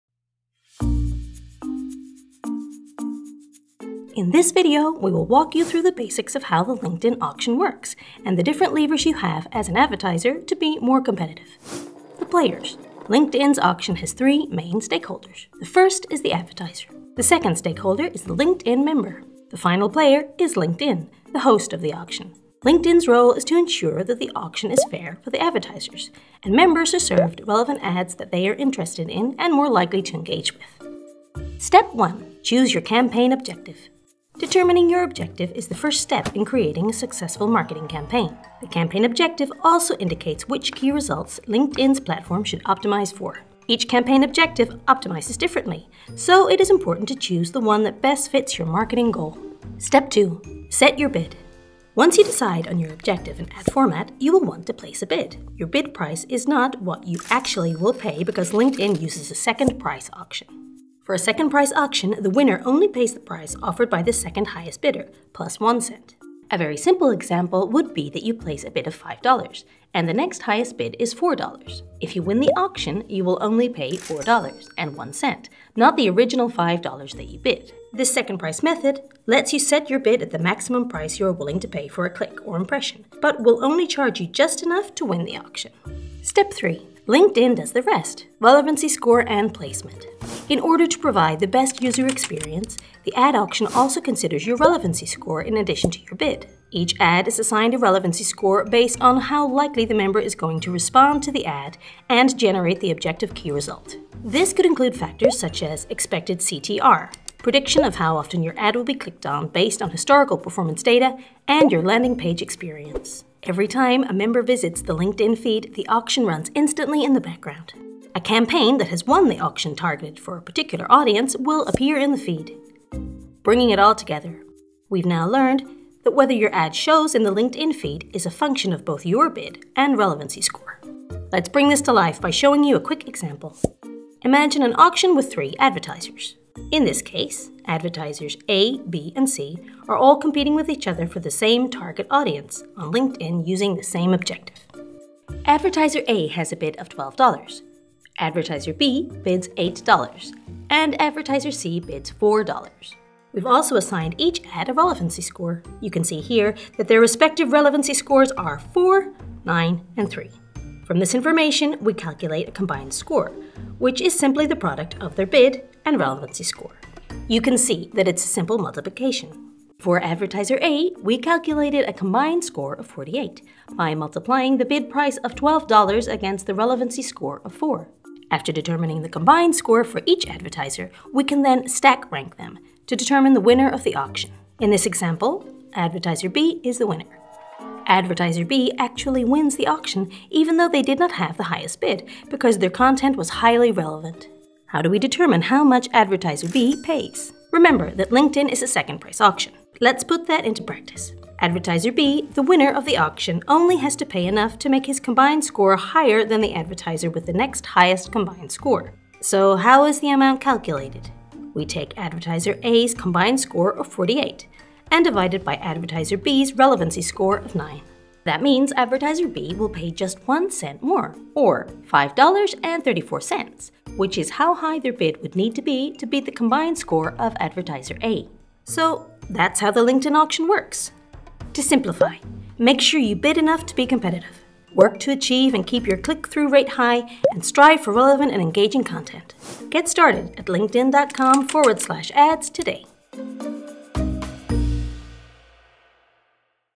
How Does the LinkedIn Auction Work?
From setting objectives to bidding, learn more about the ins and outs of LinkedIn’s advertising auction.